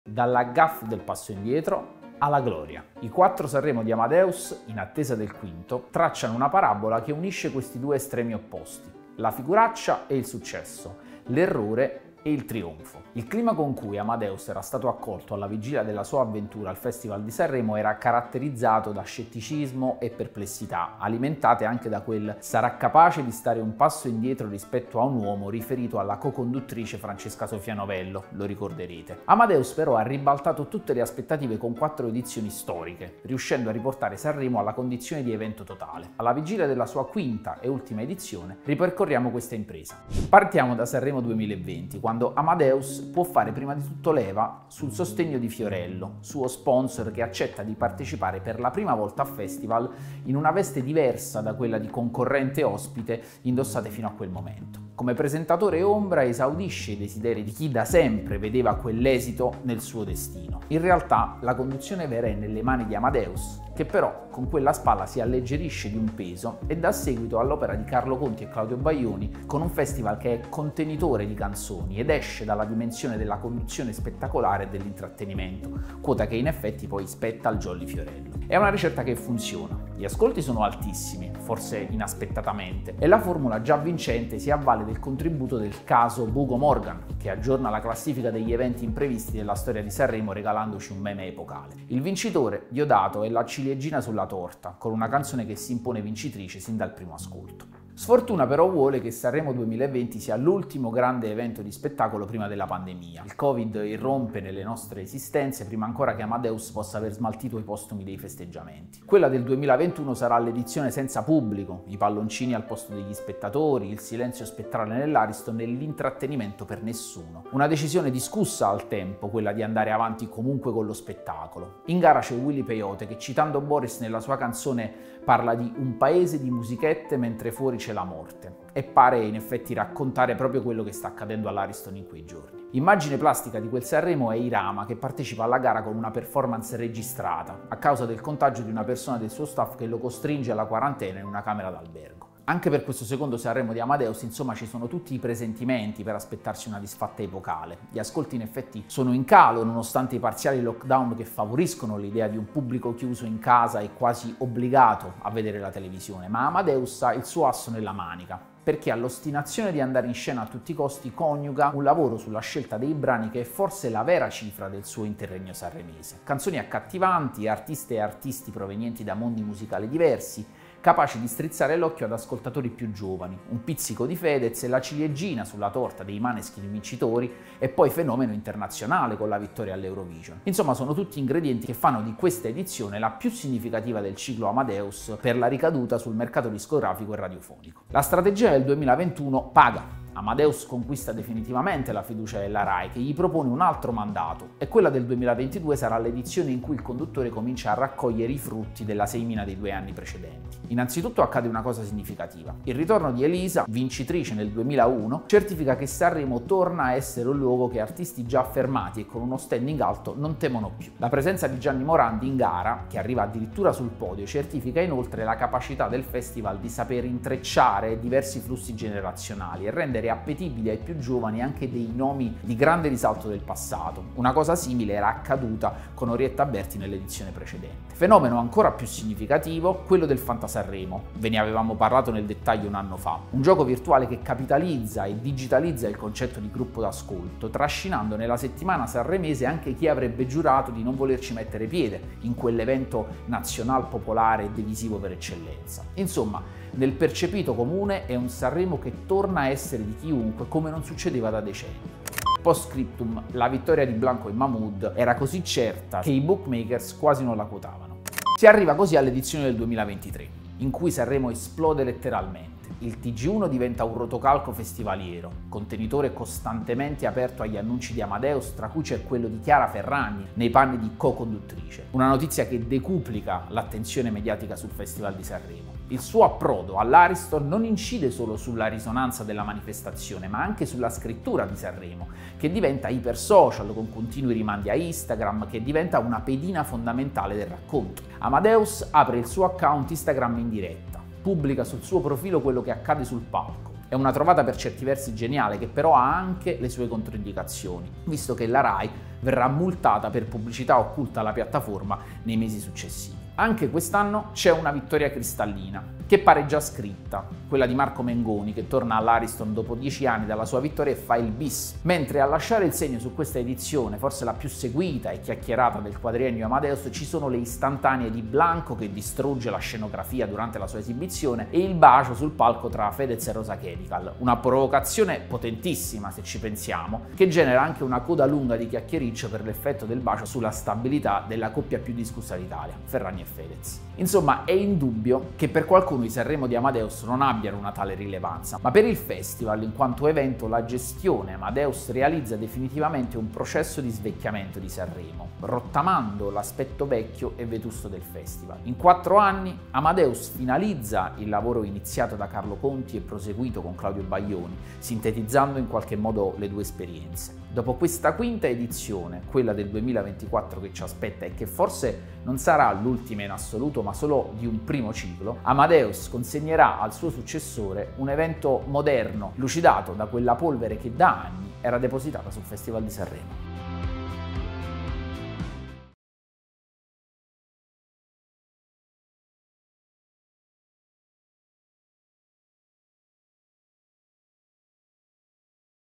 Come Amadeus ha cambiato la storia del Festival di Sanremo
Le frasi imbarazzanti su Francesca Sofia Novello vengono ricordate come il primo atto del regno di Amadeus a Sanremo. Da quelle parole, che al tempo alimentarono molti dubbi sul direttore artistico, sono trascorse quattro edizioni con cui Amadeus ha rivoluzionato il Festival di Sanremo, riportandolo alla dimensione di evento totale. Fiorello, Bugo e Morgan, il Covid e l'Ariston vuoto, I Maneskin, Morandi con Jovanotti. E ancora Blanco e Mahmood, il FantaSanremo, Mengoni e Chiara Ferragni. I primi quattro Sanremo di Amadeus, alla vigilia del quinto che dovrebbe essere l'ultimo, finalizzano il lavoro iniziato da Conti e Baglioni e segnano l'evoluzione definitiva di un evento svecchiato, lucidato da quella polvere che per troppo tempo era rimasta depositata sul festival.

0:00 - Introduzione: Dalla gaffe alla gloria
0:20 - Sanremo 2020: L'inizio della trasformazione
1:38 - Sanremo 2021: L'edizione della pandemia
2:25 - Sanremo 2022: La consacrazione del successo
3:01 - Sanremo 2023: L'esplosione del fenomeno social
4:03 - Prospettive per il 2024: L'ultima edizione di Amadeus?
5:32 - Conclusioni: L'eredità di Amadeus a Sanremo